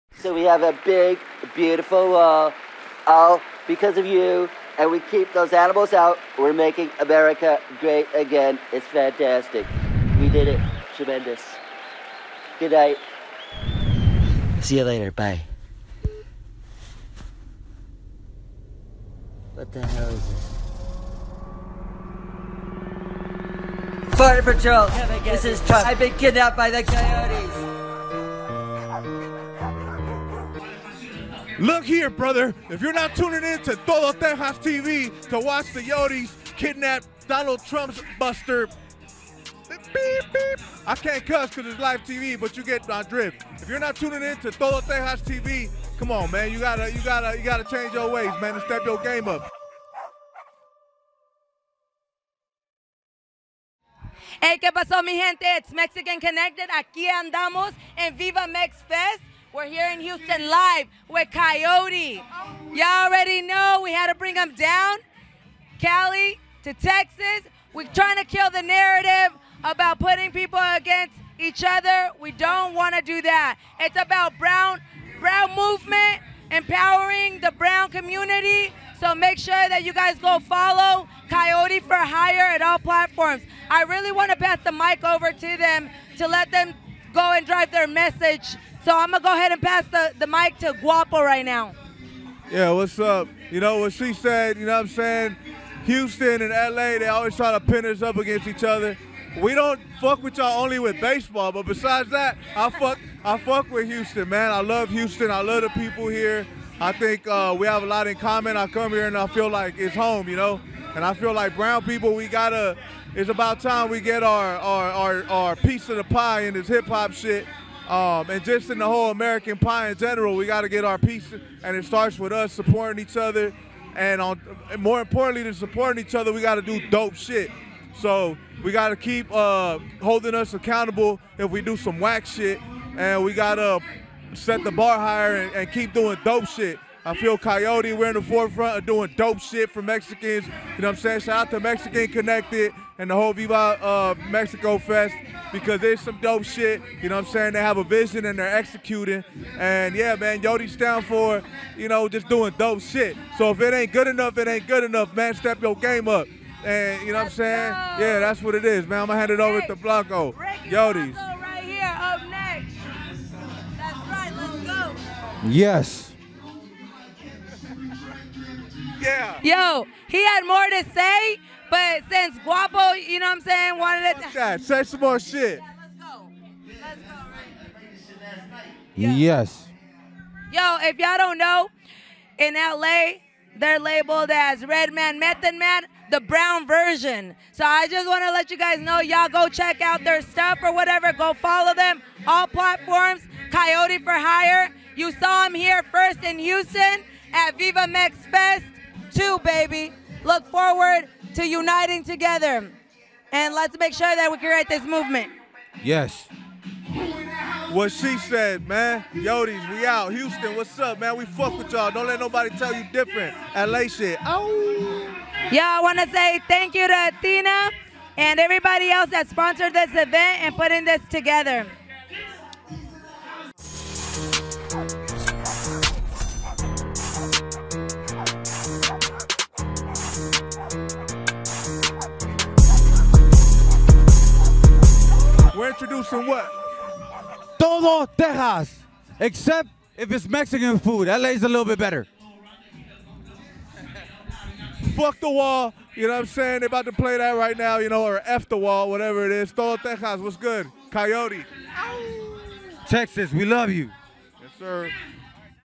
Coyote For Hire Rap Artist
Mexican Connected Host of Total Tejas TV Interviews Coyote For Hire Rap Artist